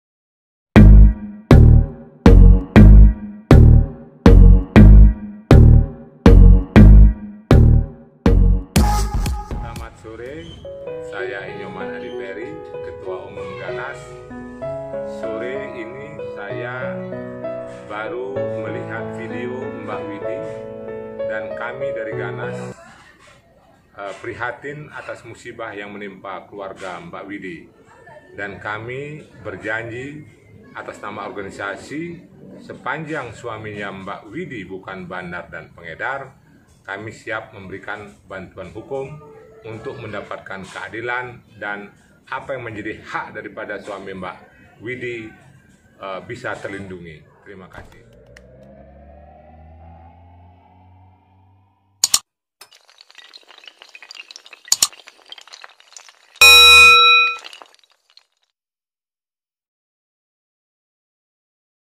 I Nyoman Adi Peri SH, Ketum GANNAS Prihatin Terkait Penangkapan DS
I Nyoman Adi Peri SH, Ketua Umum Gerakan Anti Narkoba Nasional (GANNAS) Prihatin Terkait Penangkapan DS . Pemakai narkoba adalah korban yang patut dibela hak-haknya, beda dengan pengedar dan bandar.
GANNAS siap bela yang menjadi korban narkoba.